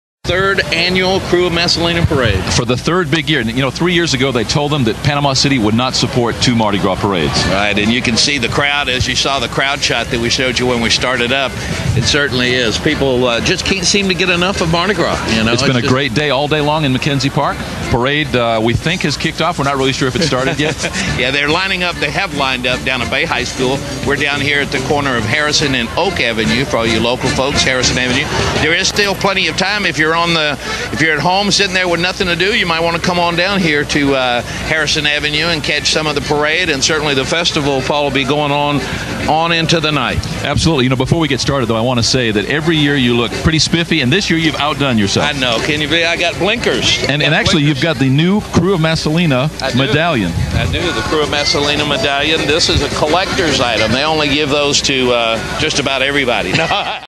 PGP tom najjar mardi gras clip for web site rev.wmv
Promotional Video Clip for The Goulding Agency in Chipley, Florida.
Live broadcast of the Krewe of Massalina Mardi Gras Parade in Panama City Florida.  Produced by the Goulding Agency.